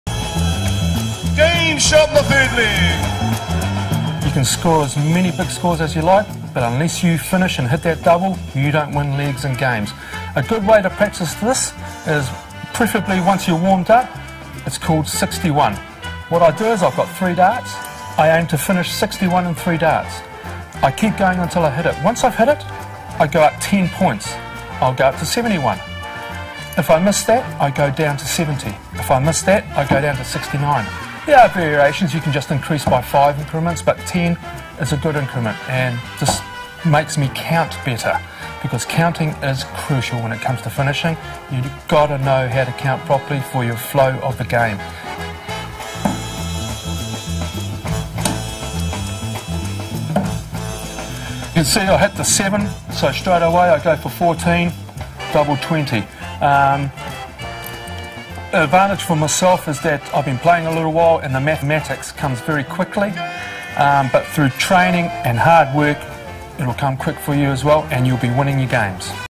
Rob Szabo Number Practice for Darts
Rob Szabo Shot! Ambassador give tips on improving your counting and getting your maths right when playing darts.